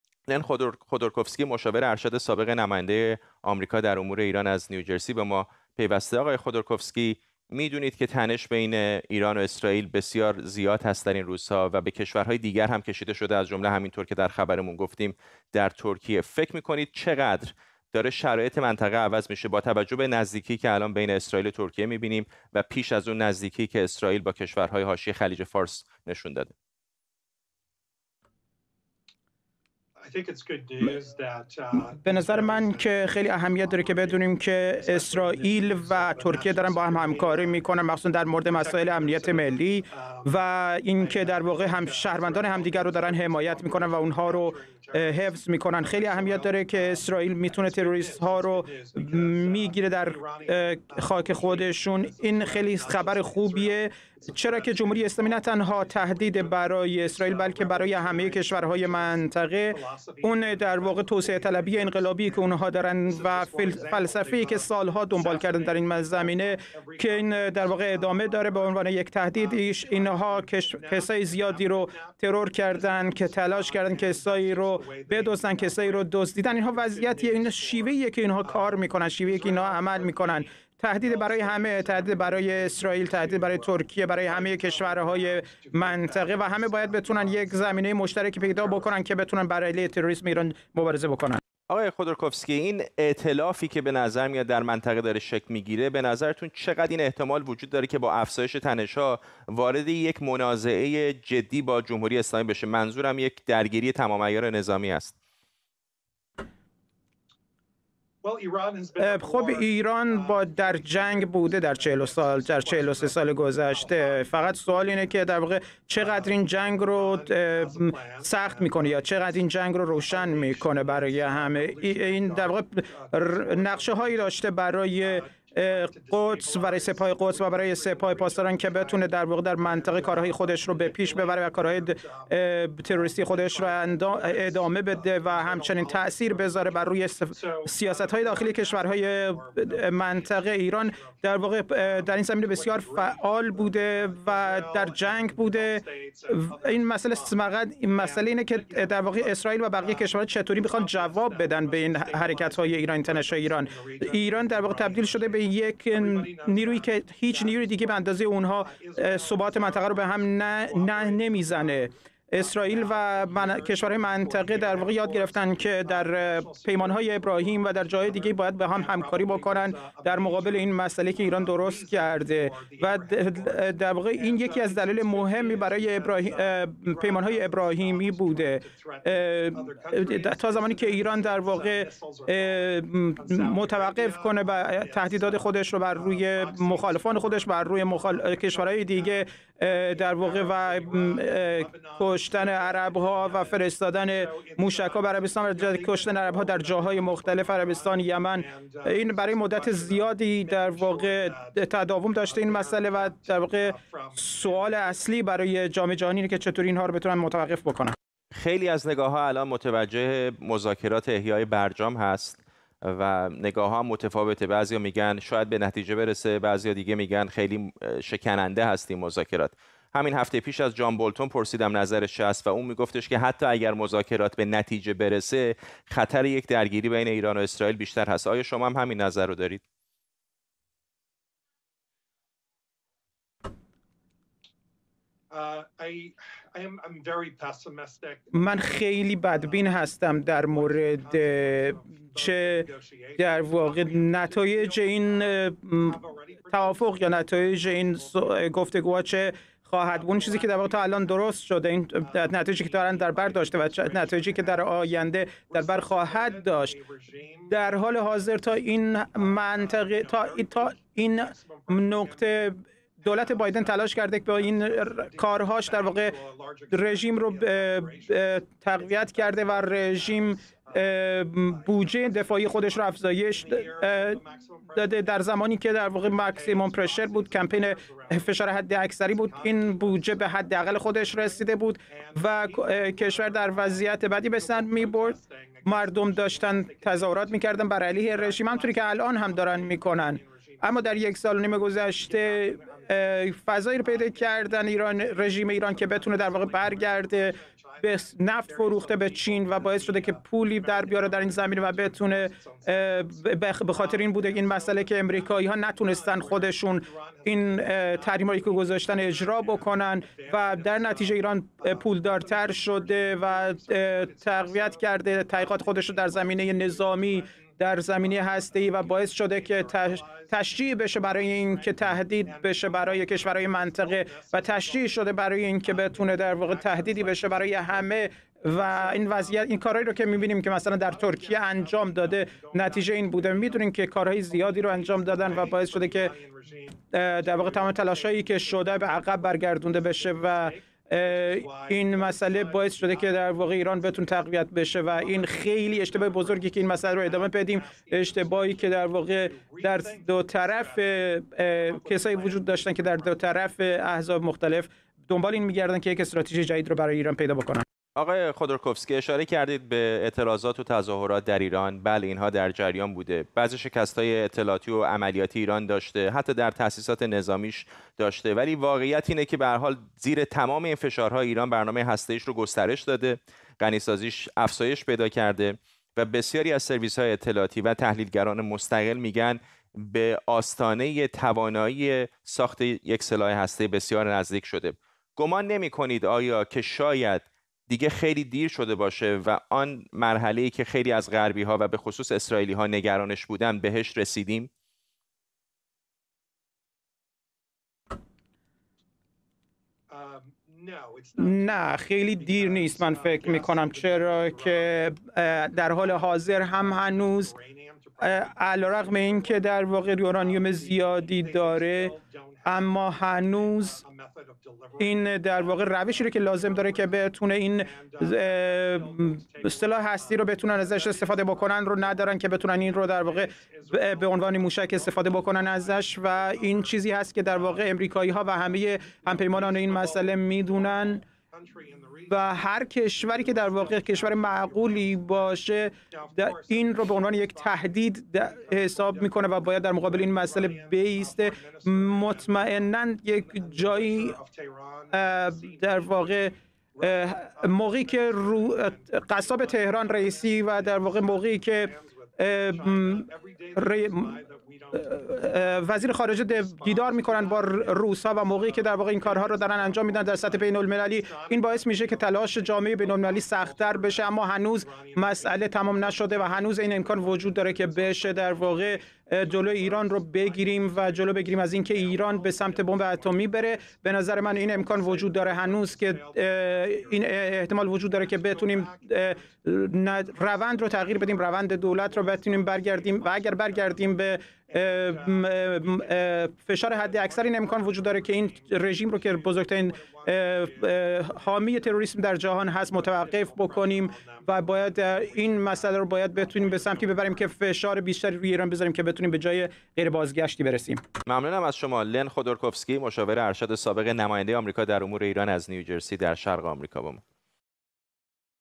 گفت‌وگو با لن خودورکوفسکی، مشاور ارشد سابق نماینده آمریکا درباره‌ی دستگیری مظنونان ایرانی در ترکیه
گفت‌وگو با لن خودورکوفسکی، مشاور ارشد سابق نماینده آمریکا در امور ایران درباره‌ی دستگیری مظنونان ایرانی در ترکیه به اتهام دست داشتن در طرح ترور شهروندان اسرائیلی